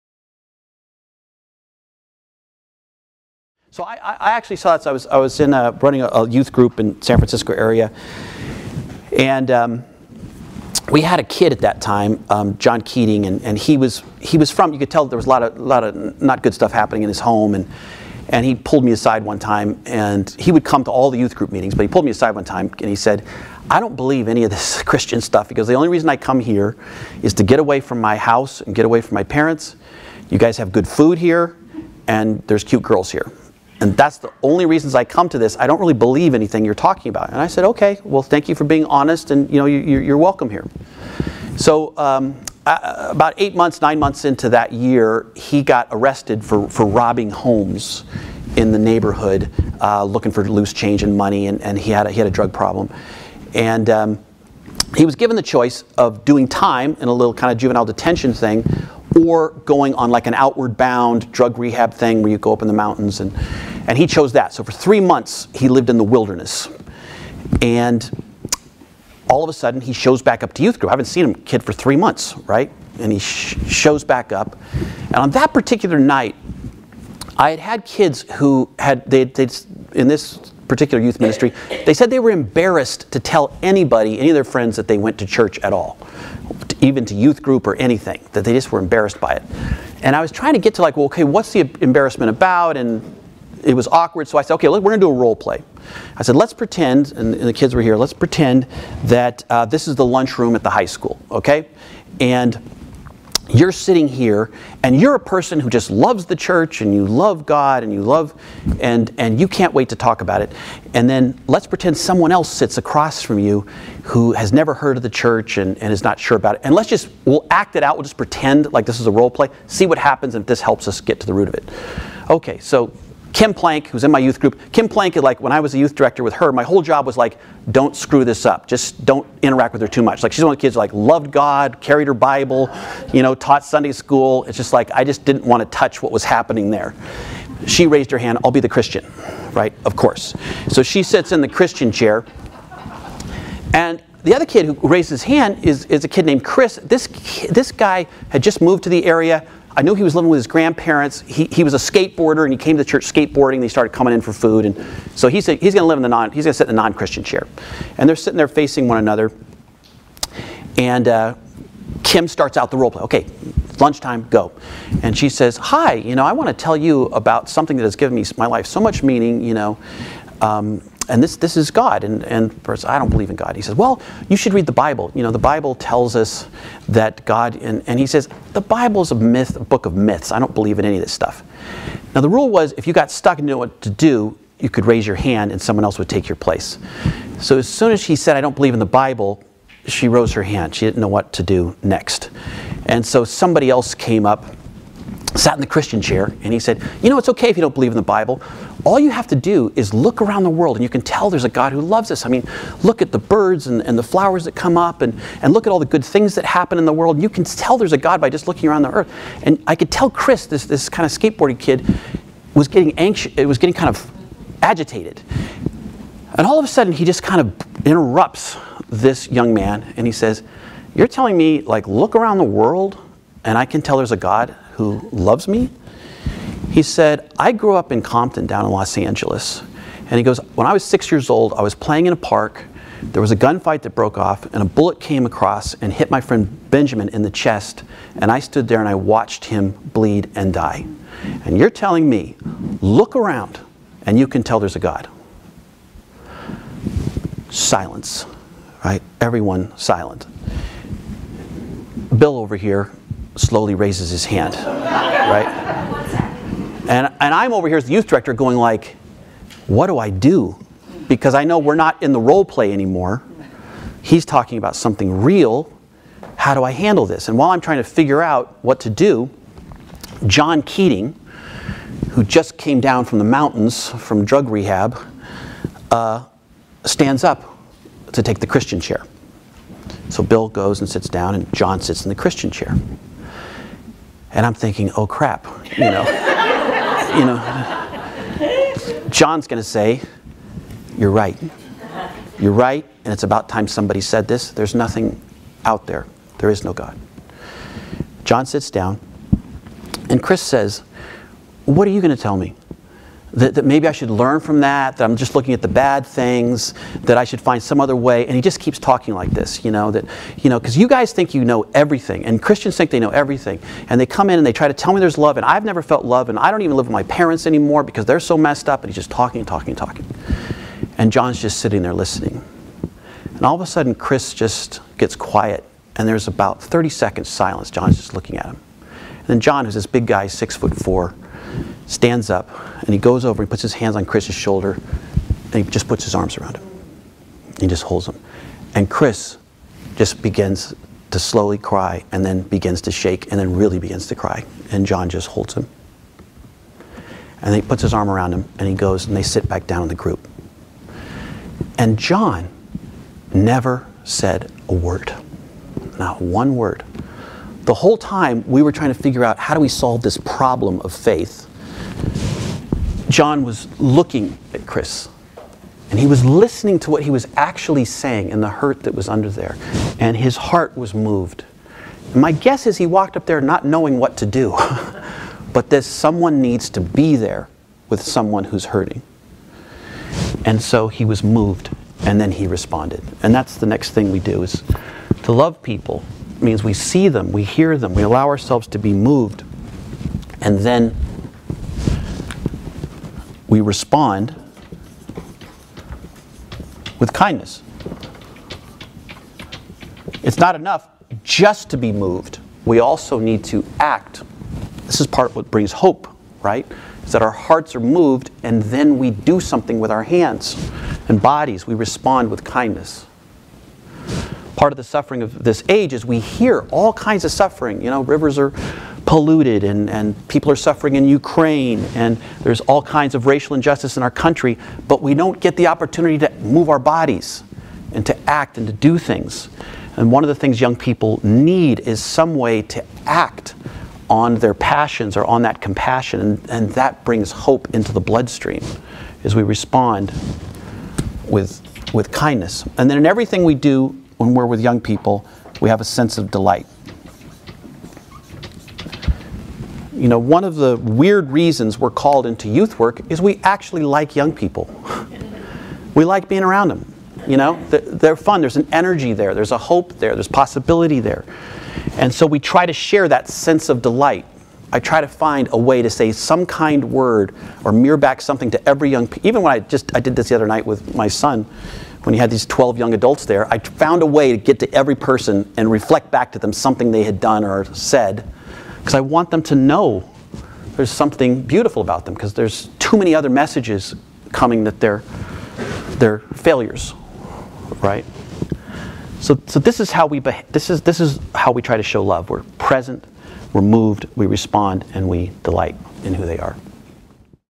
How to Help Youth Listen to Each Other
Tending the Adolescent Soul: Offering Hope in an Age of Despair with Mark Yaconelli, March 25, 2023. This clip is about how to help youth listen to each other.